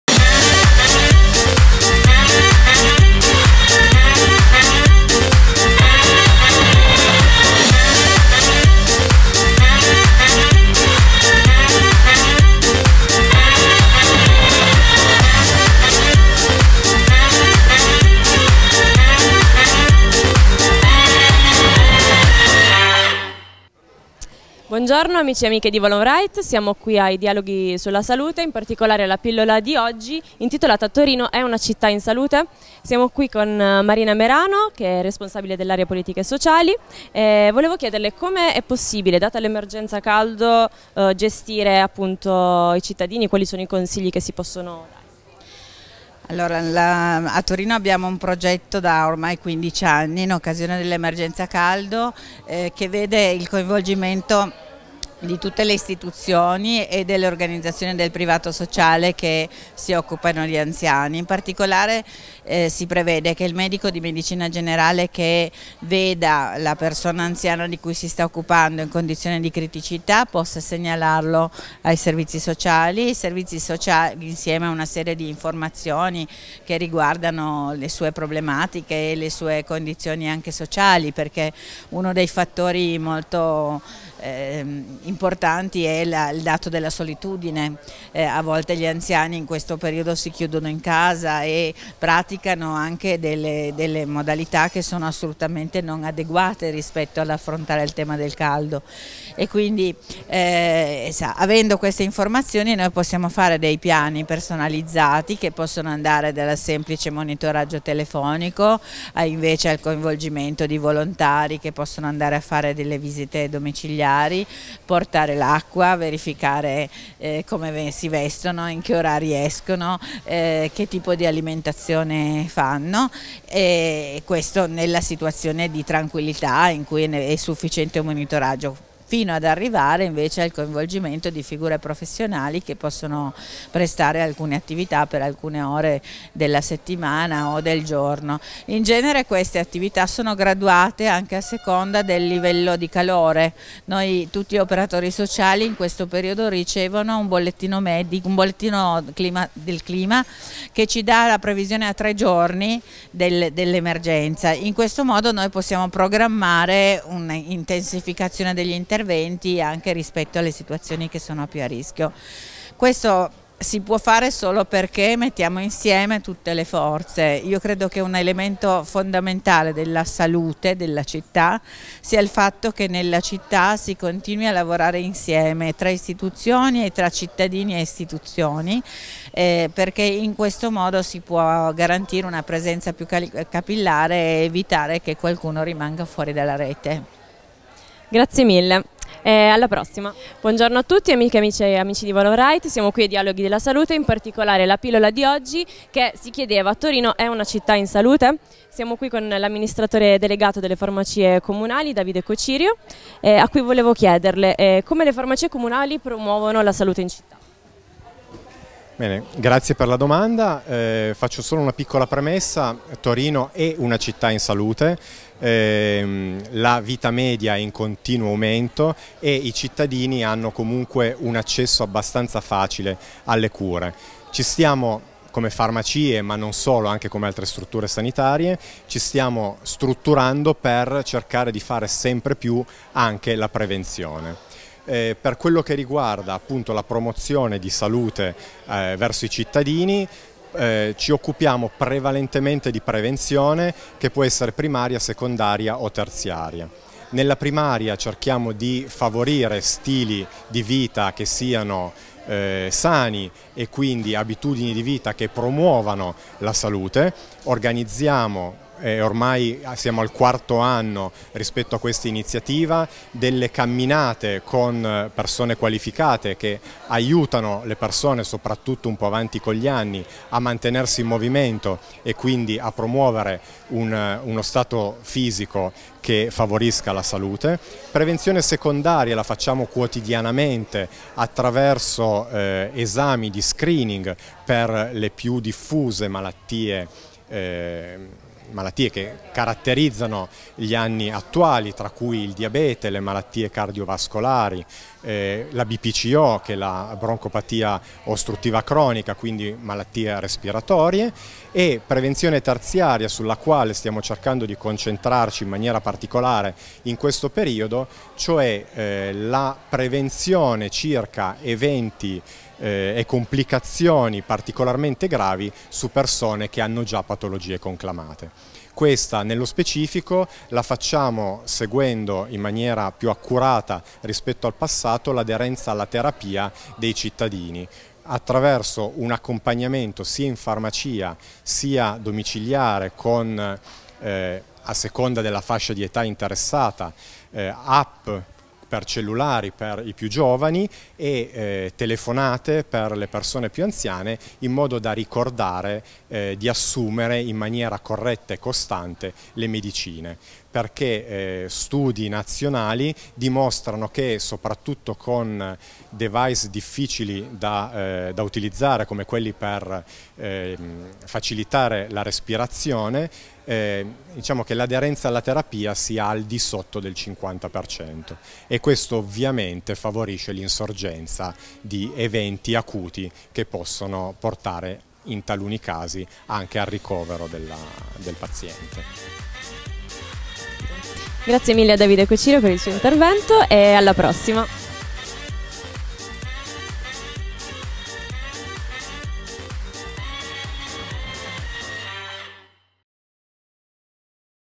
Dialoghi sulla Salute 2019 - intervista a Marina Merano e Davide Cocirio
5 luglio, quinta giornata dei Dialoghi sulla Salute!
La pillola di oggi ha avuto come tema “Torino è una città in salute?” ne abbiamo parlato con l’Assessora al Welfare Sonia Schellino, al Direttore della Divisione Servizi Sociali del Comune di Torino Monica Lo Cascio, al Direttore Generale ASL Torino Valerio Fabio Alberti, all'Amministratore Delegato di Farmacie Comunali Torino Davide Cocirio.
Al termine, noi di Associazione Volonwrite abbiamo avuto il piacere di intervistare i relatori Marina Merano e Davide Cocirio.
- intervista a cura di Erica Zingaro;
- riprese di Fabio Rossignoli;
- montaggio di Federico Stella.